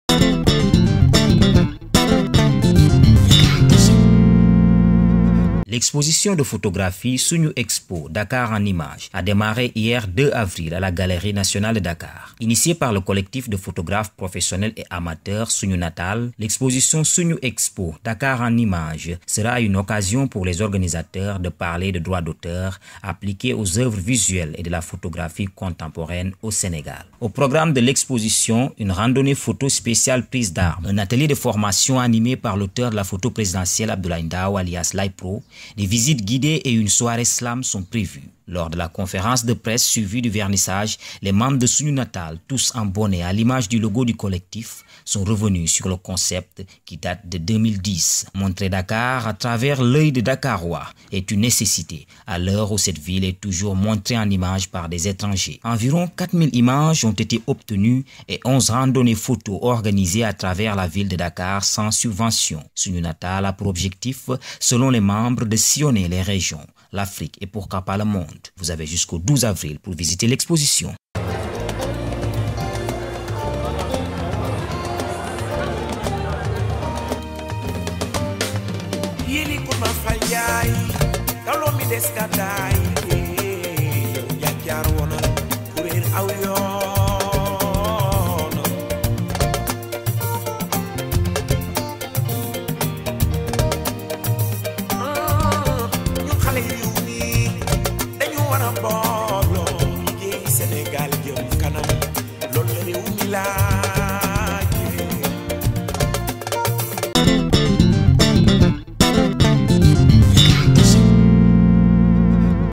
Vernissage "Sunu Expo-Dakar en Images" de SUNU NATAL
L’exposition de photographies « Sunu Expo-Dakar en images » a démarré hier 02 avril à la Galerie Nationale de Dakar. Initiée par les membres du collectif de photographes professionnels et amateurs SUNU NATAL, l’expo  « Sunu Expo-Dakar en images » sera une occasion pour les organisateurs de parler de droits d’auteurs appliqués aux œuvres visuelles et de la photographie contemporaine au Sénégal.
Au programme de l’exposition, une randonnée de photo spéciale prise d’armes, un atelier de formation animé par l’auteur de la photo présidentielle Abdoulaye Ndao alias Laye Pro, des visites guidées et une soirée slam  sont prévus.
Lors de la conférence de presse suivie du vernissage, les membres de SUNU NATAL, tous en bonnets à l’image du logo du collectif sont revenus sur le concept qui date de 2010. Montrer Dakar à travers les yeux de dakarois est une nécessité à l’heure où cette ville est montrée en image par des étrangers.
Environs 4000 images ont été obtenus et 11 randonnées photos organisées à travers la ville de Dakar sans subvention. SUNU NATAL a pour objectifs, selon les membres, de sillonner les régions du Sénégal,  l’Afrique et pourquoi pas le monde. Vous avez jusqu’au 12 avril pour visiter l’exposition